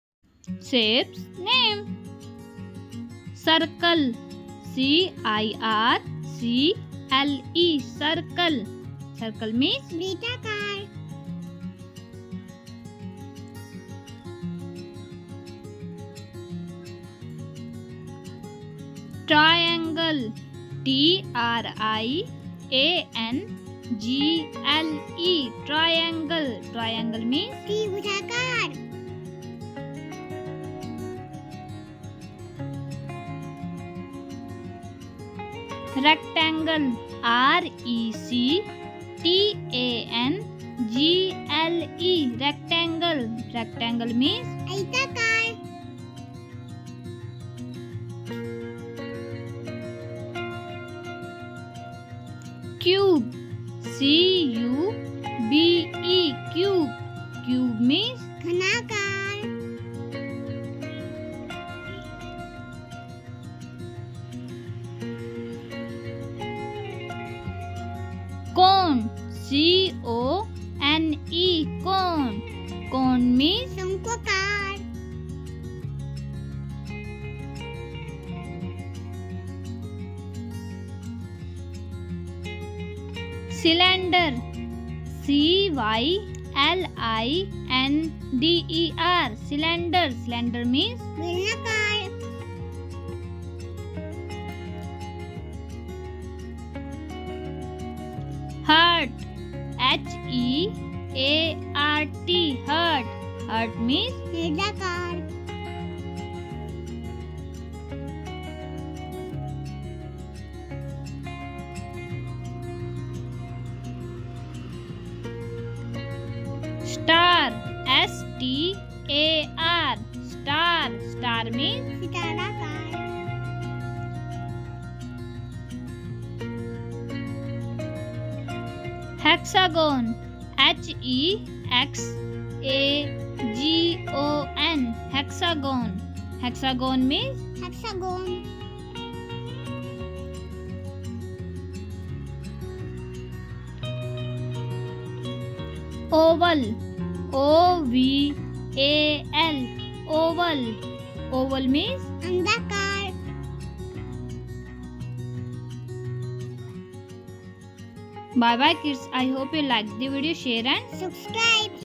Shapes Rhymes !! Shapes with Spelling & Hindi !! Shapes song kids !! Mini TV kids
hello friends welcome to mini TV kids
learning is always fun with mini TV
Shapeslearning listening and writing Shapes Hindi and English with spellings and song
I hope you like this video enjoy this video and funny video